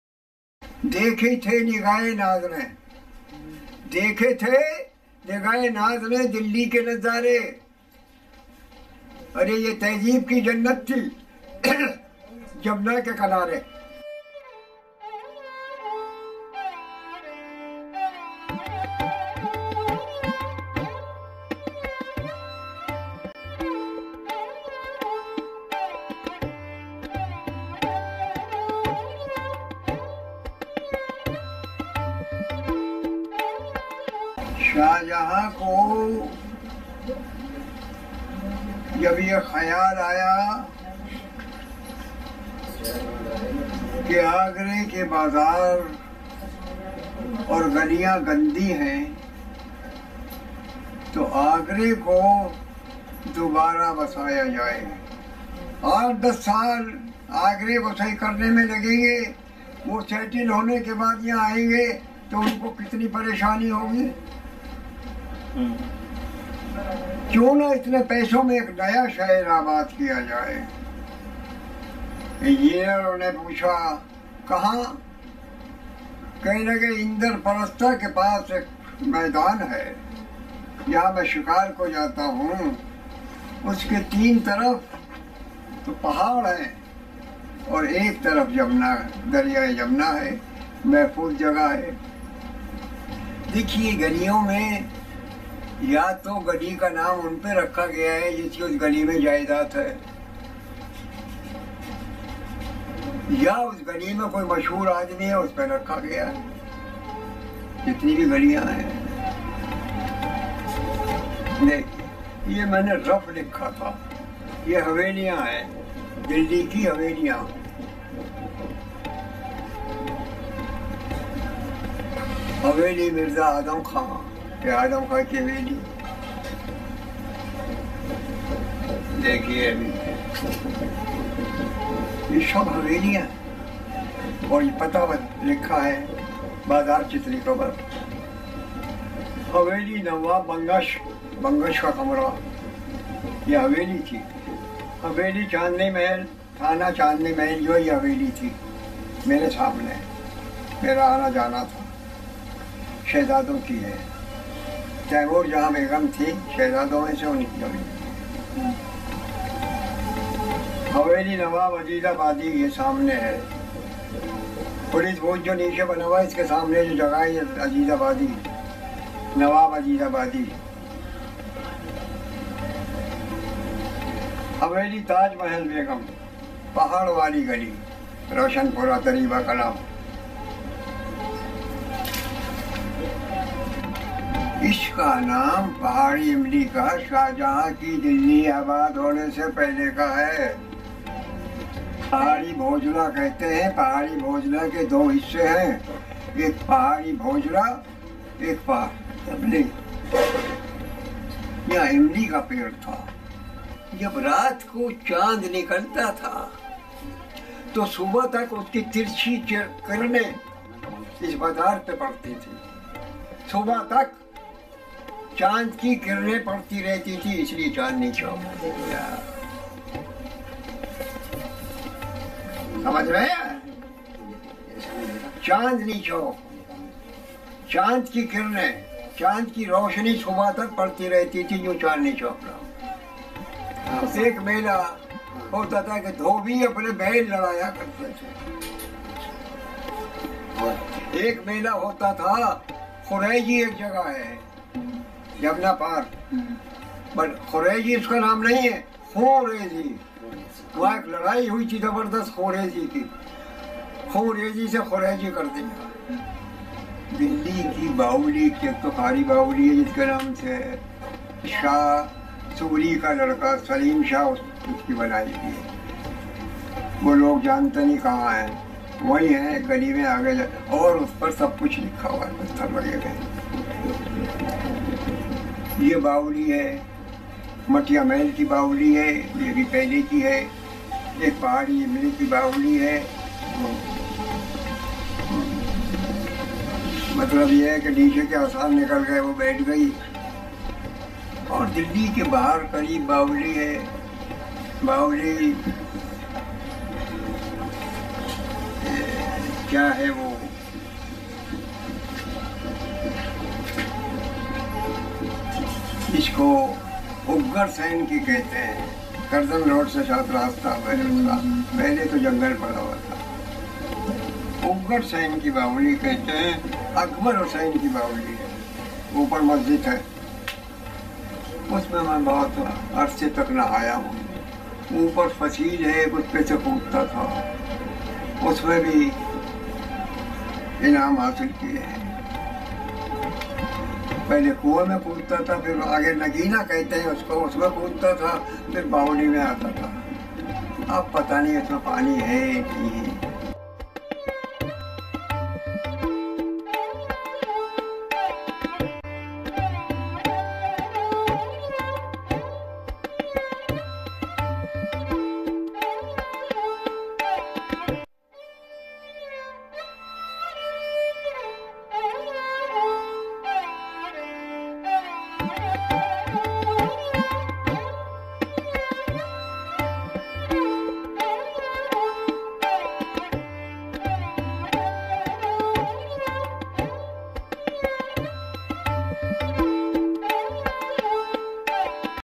The History of Old Delhi ; The diary of Nizam Beg Changezi
A short film based on the experiences of Nizam Beg Changezi, a 104 Years old citizen of Chandni Chowk.